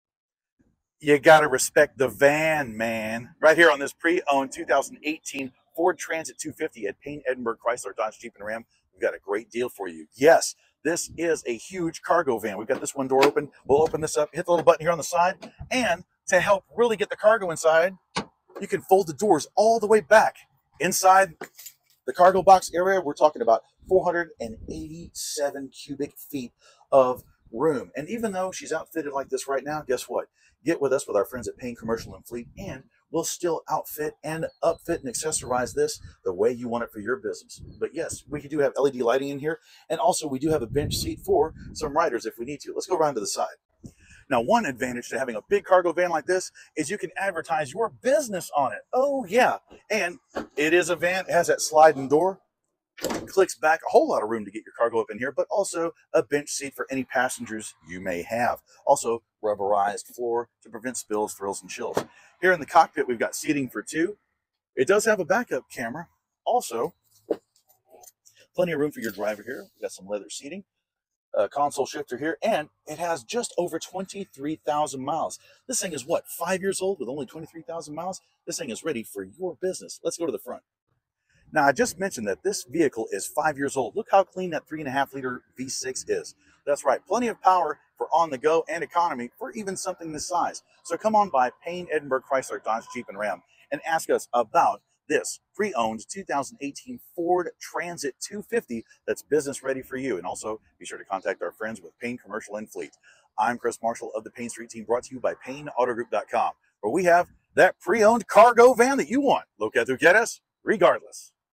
2018 Ford Transit Van | Payne Edinburg CDJR | Edinburg, Texas
‼️Check out our latest Lot Stop with Payne Street Team Chris Marshall as we take a closer look at this brand new 2018 Ford Transit Van available now at Payne Edinburg Chrysler Doge Jeep & RAM‼️

+35 Financial Lenders
+Hassle-Free Approvals